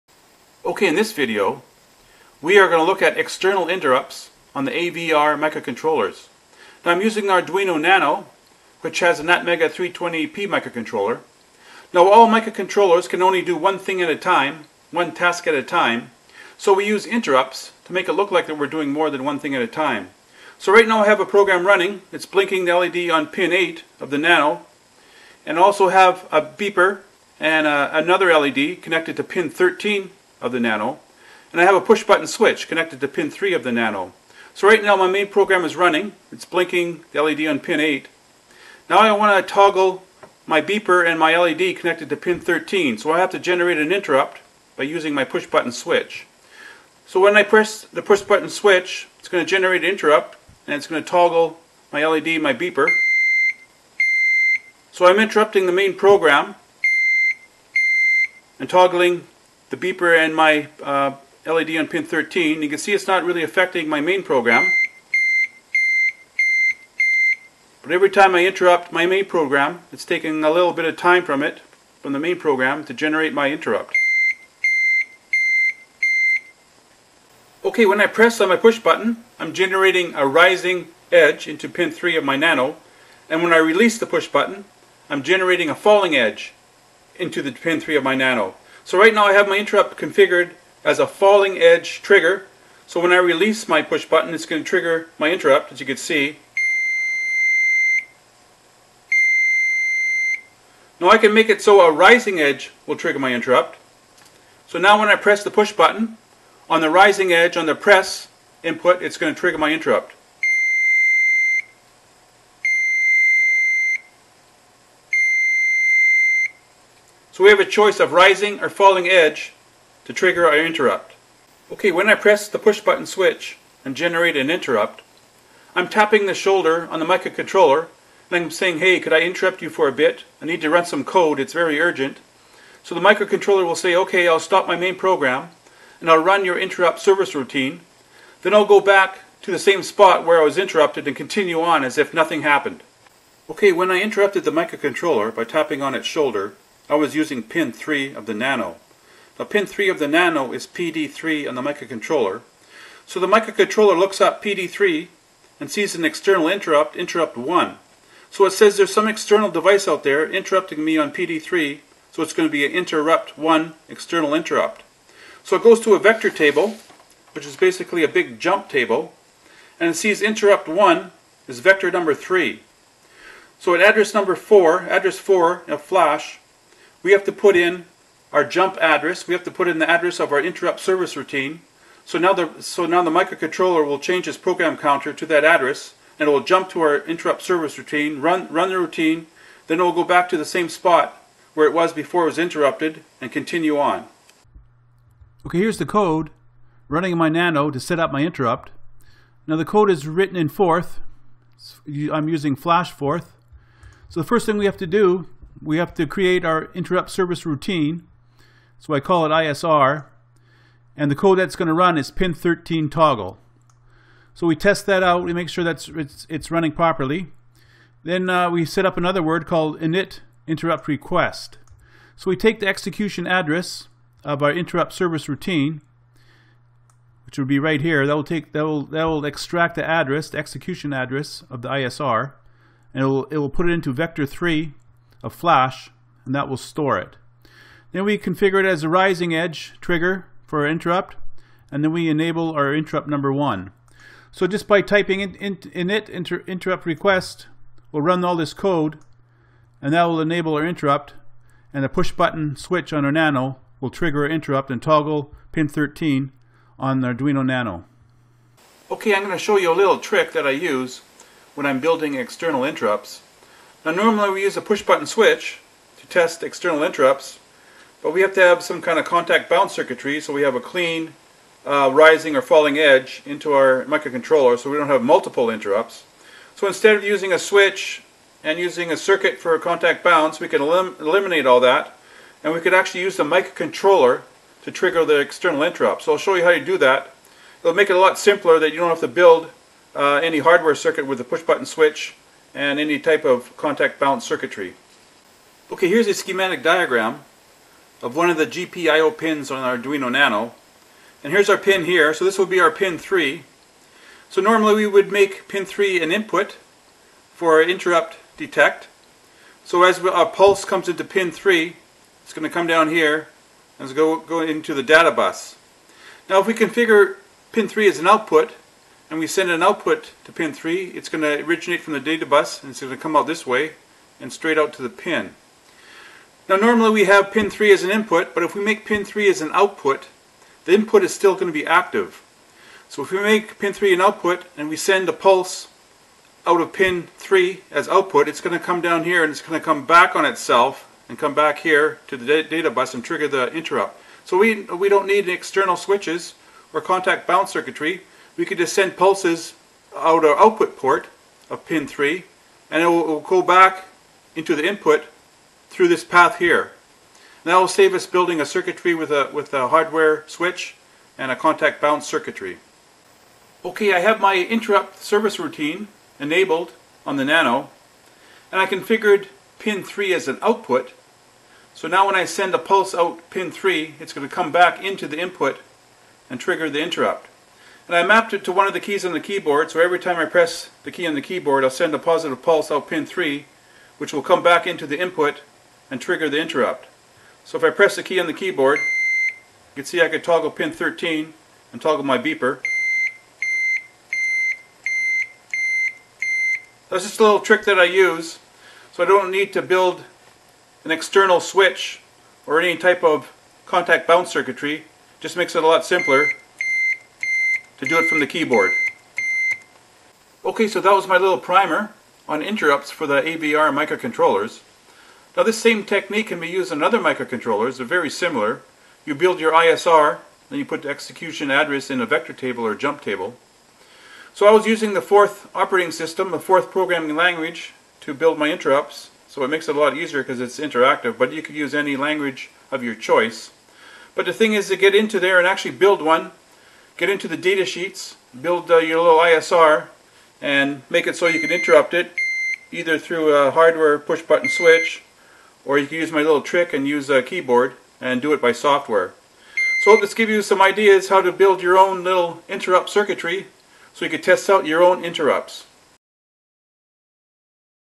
Arduino External Interrupts / AVR
This video will describe the workings of an external interrupt running on an Arduino Nano ATmega328p microcontroller.